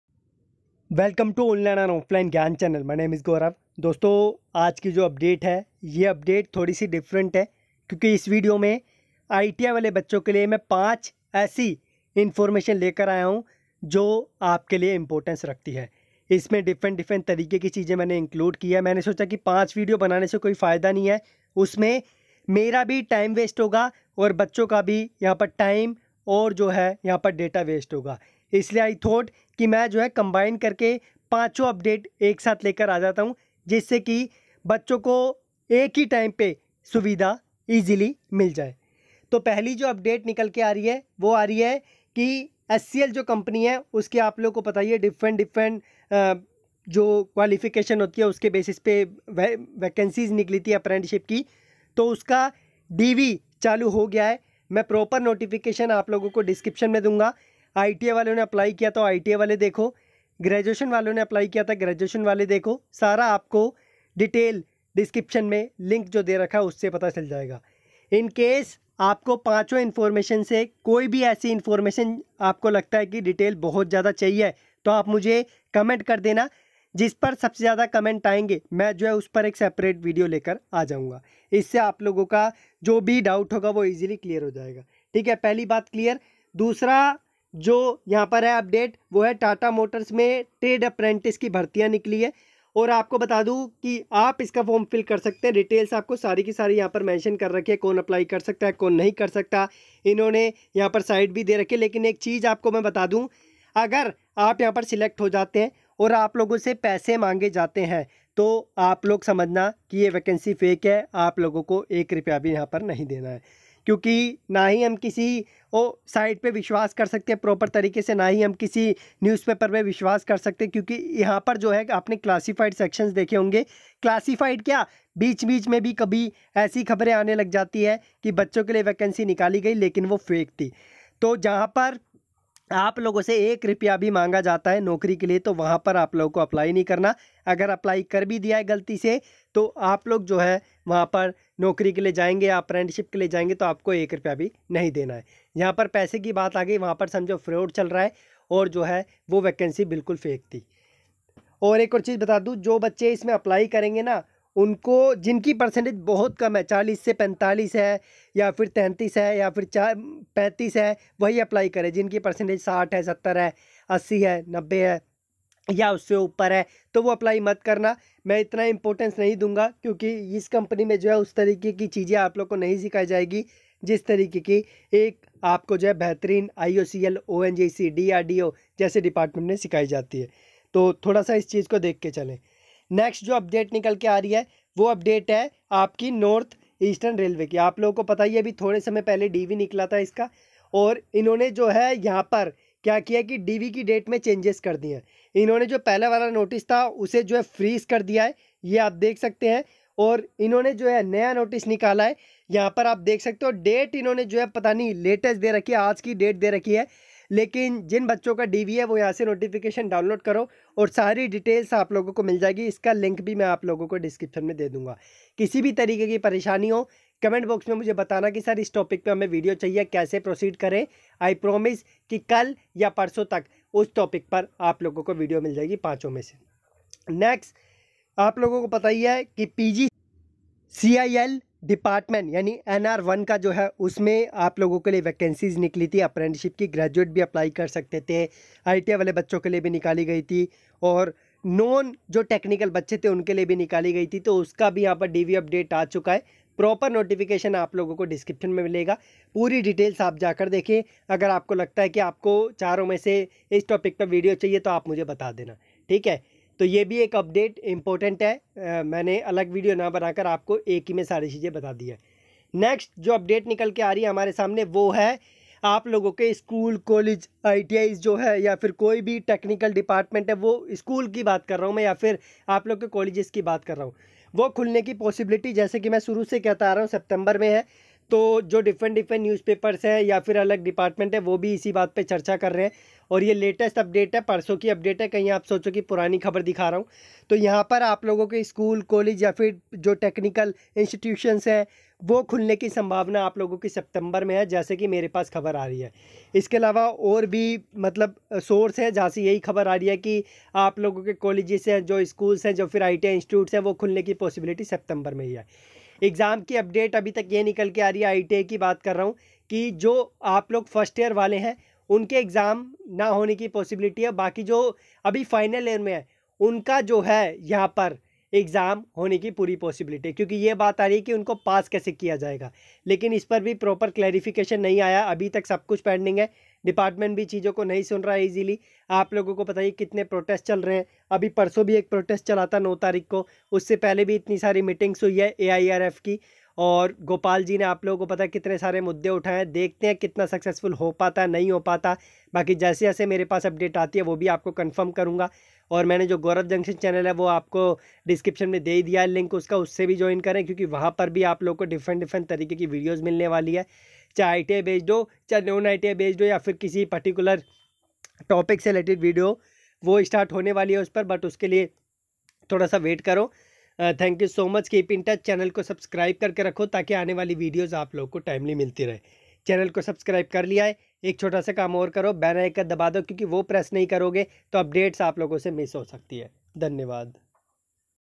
ITI से जुड़ी महत्वपूर्ण खबरें || ITI College, ITI Job, ITI Naukri, Latest DV, Apprentice Merit List.
ITI important News and Updates for August month 2020

Thanks,
Online and offline gyan